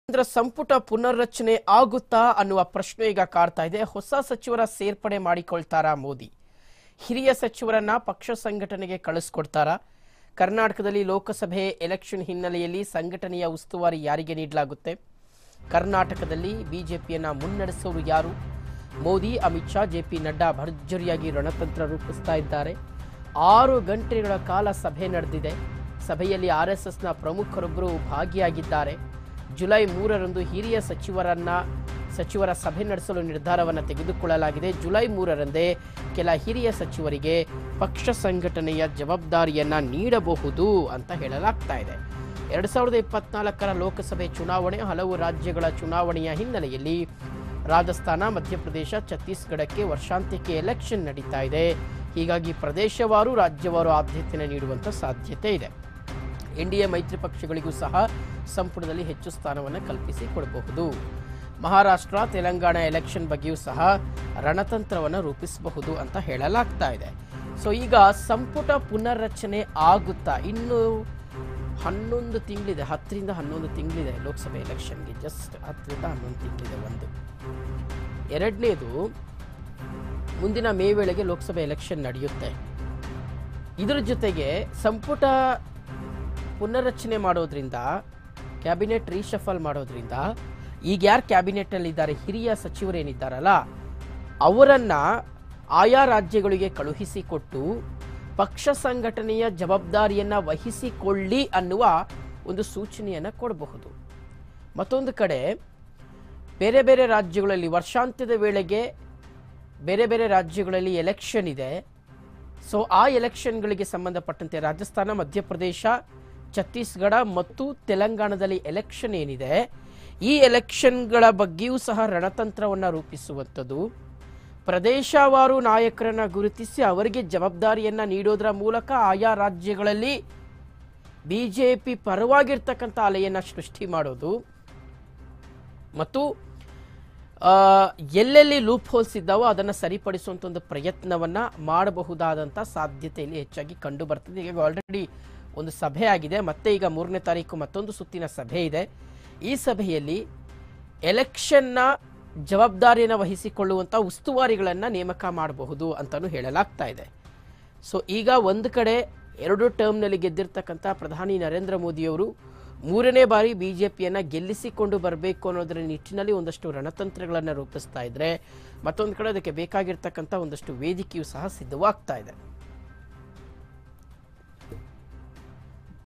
ಶೀಘ್ರವೇ ಕೇಂದ್ರ ಸಂಪುಟ ಪುನರ್ ರಚನೆ ಆಗುತ್ತಾ..? ಹೊಸ ಸಚಿವರ ಸೇರ್ಪಡೆ ಮಾಡಿಕೊಳ್ತಾರಾ ಮೋದಿ..?
Btv News Kannada - ಶೀಘ್ರವೇ ಕೇಂದ್ರ ಸಂಪುಟ ಪುನರ್ ರಚನೆ ಆಗುತ್ತಾ..? ಹೊಸ ಸಚಿವರ ಸೇರ್ಪಡೆ ಮಾಡಿಕೊಳ್ತಾರಾ ಮೋದಿ..?